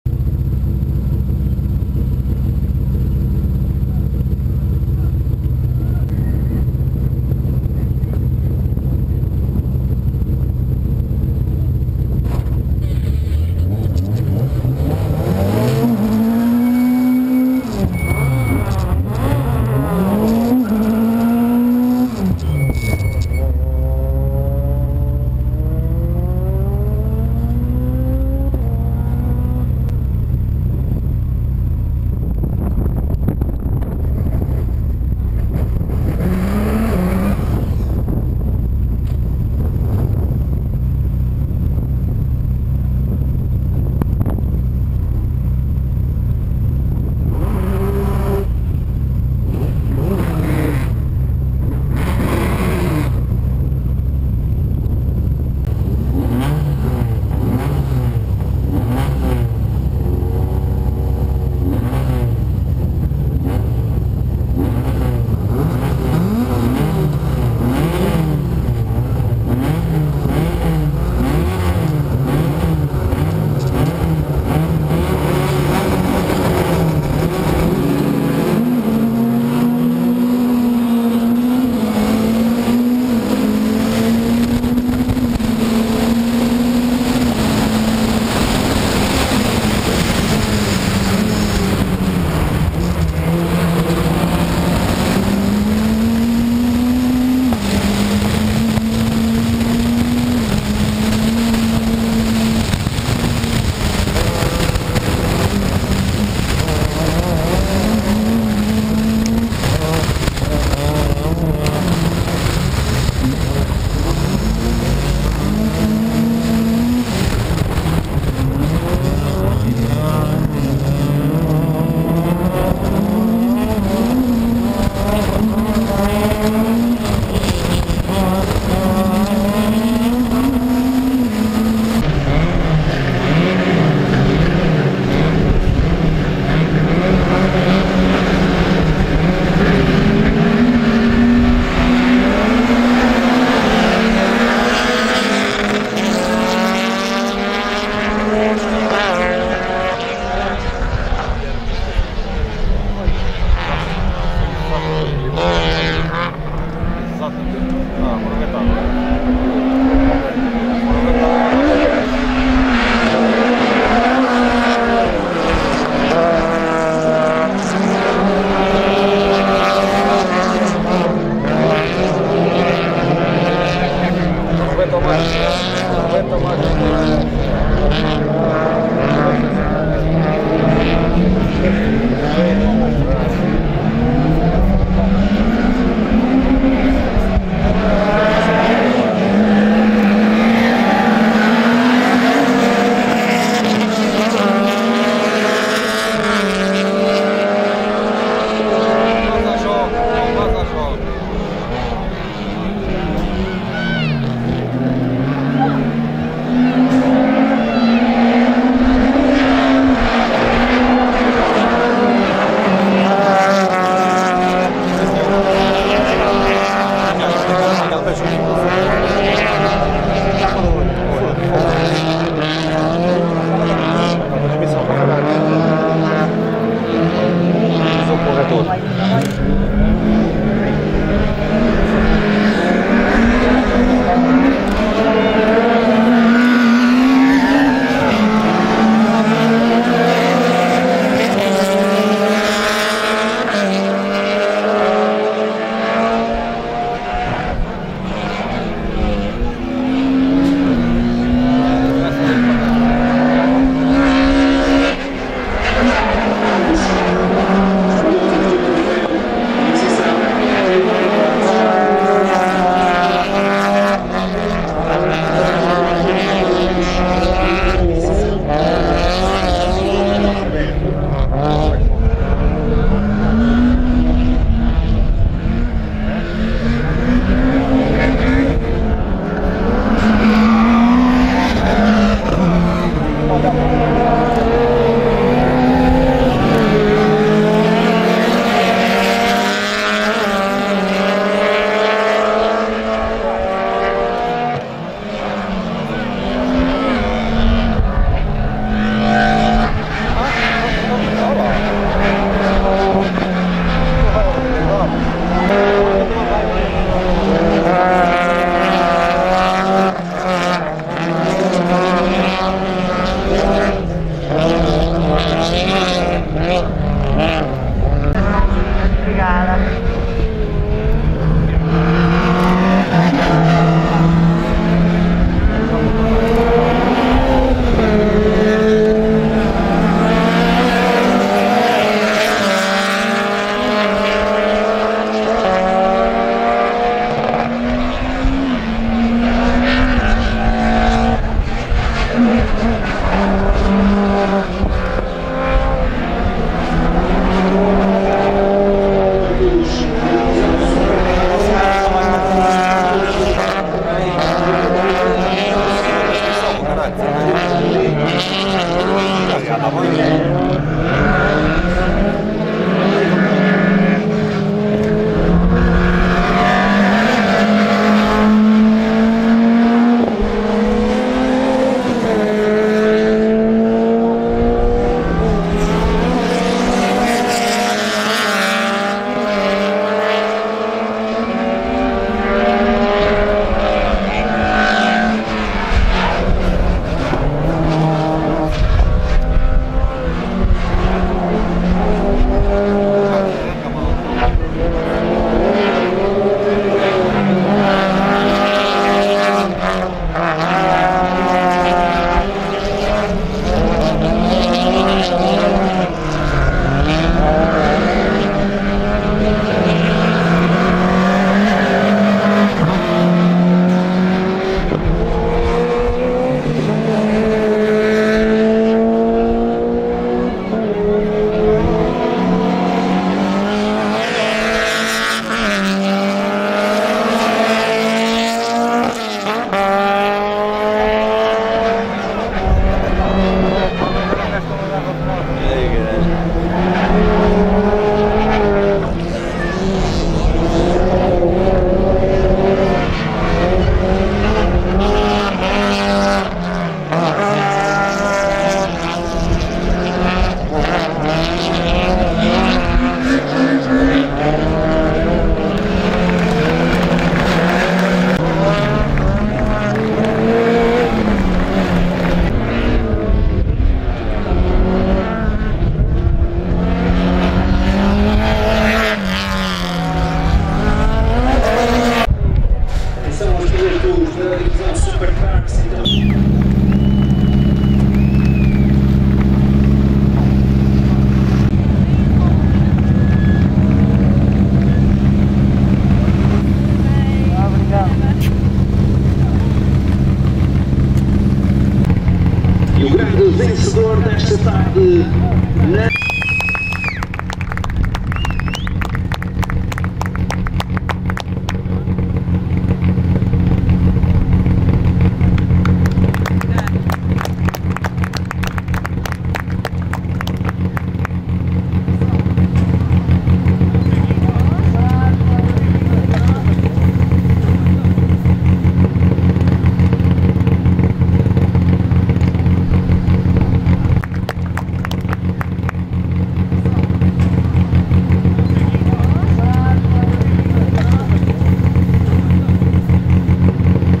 2014/05 Super 2000
Montalegre recebeu a caravana do Ralicross, a 26 e 27 de Julho, aqui ficam as imagens da final da Super 2000